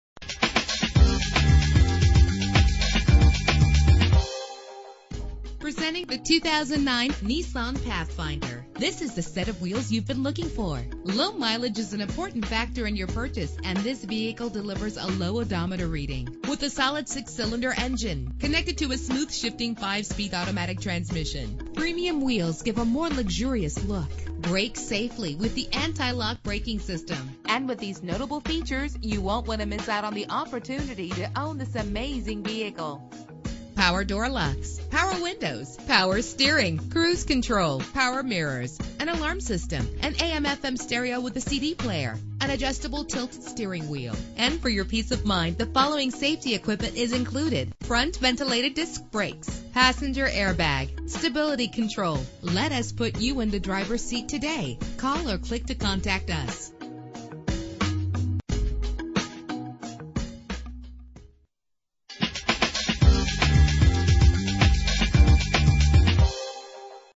2009 Nissan Pathfinder - Jackson Heights NY
Year: 2009
Make: Nissan
Model: Pathfinder
Trim: SE
Engine: 4.0 liter 6 cylinder 24 valve
Transmission: 5-Speed Automatic
Color: Navy Blue
Mileage: 6643
Address:
7415 Northern Blvd